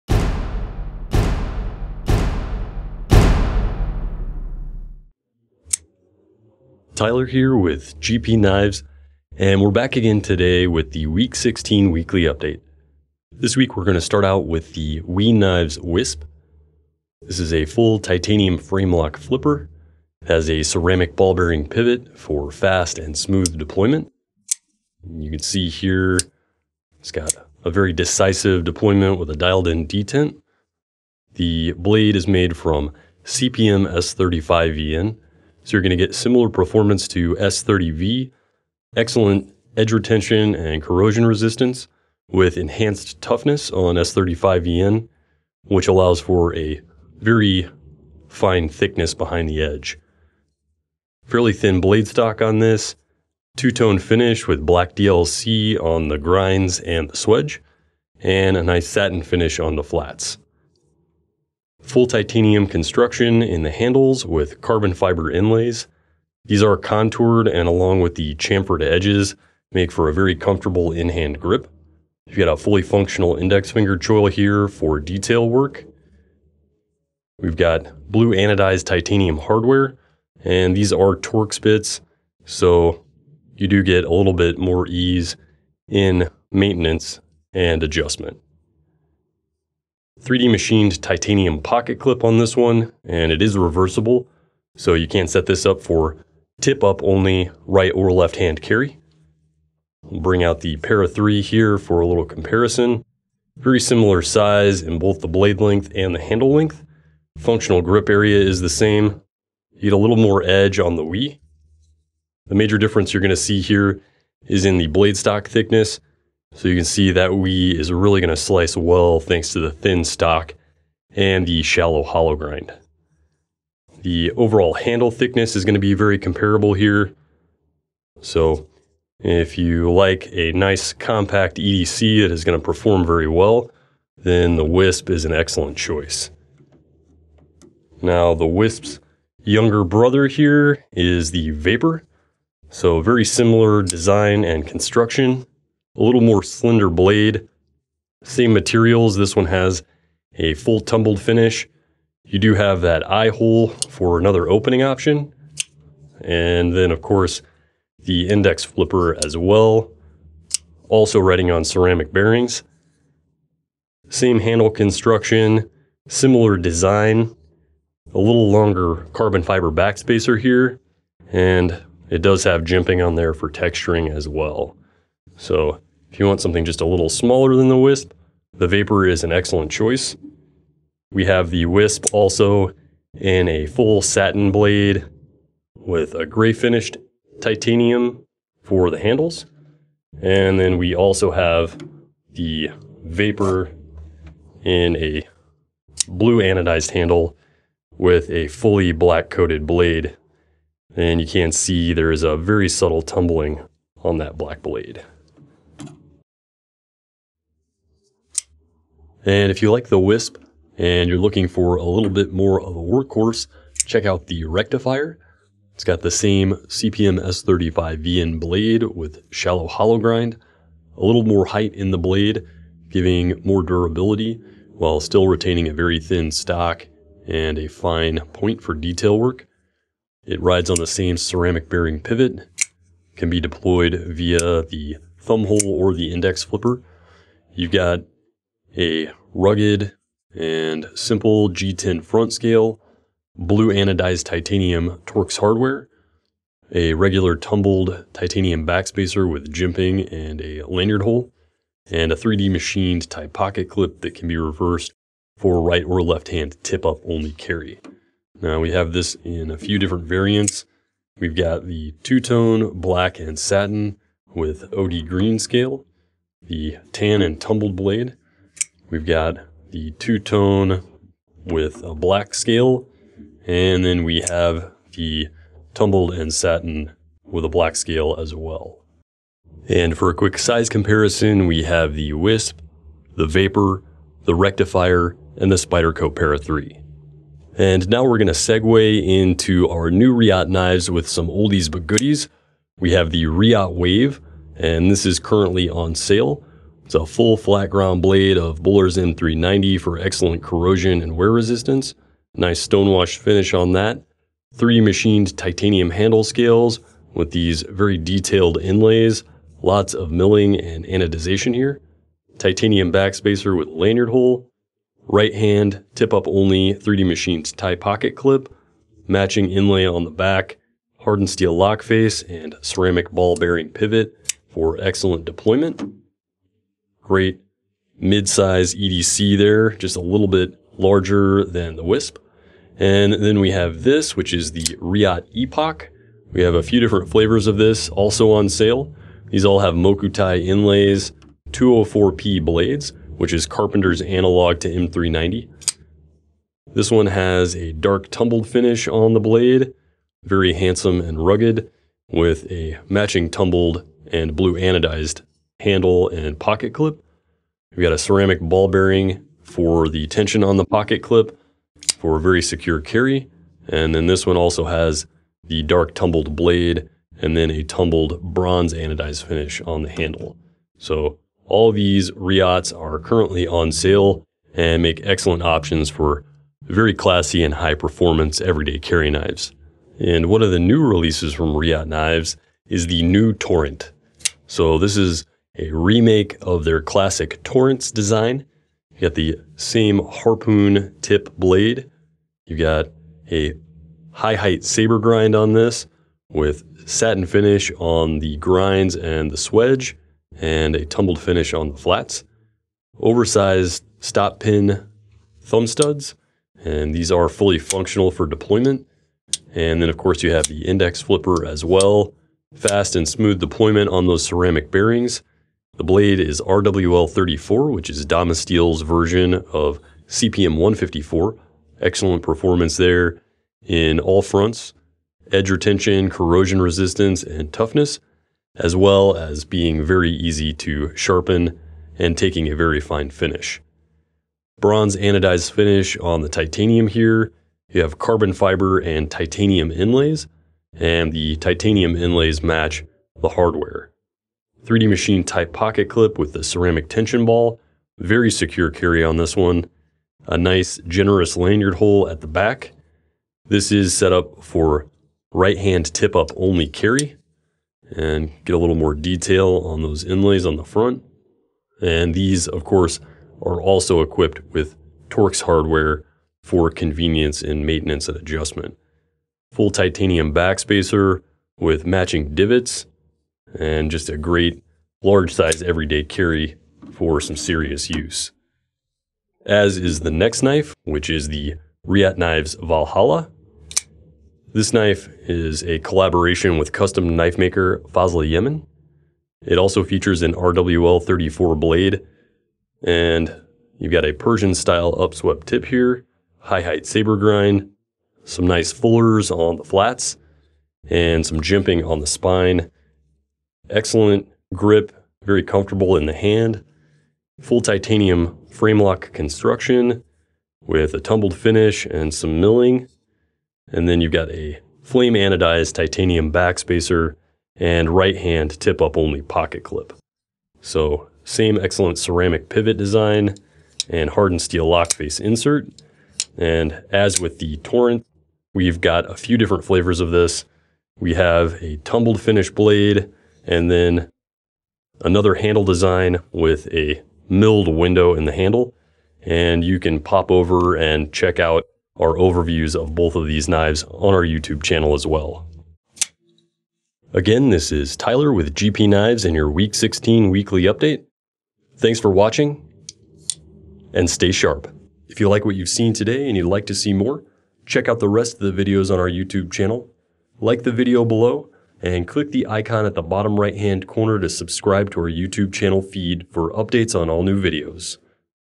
Weekly Update Week 16 - 2018: WE Knives Wisp, Vapor & Rectifier Reate Valhalla, New Torrent & More
Weekly Update Week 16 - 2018: New WE Knives S35VN Flippers -

ON SALE!!! The Reate Wave (M390)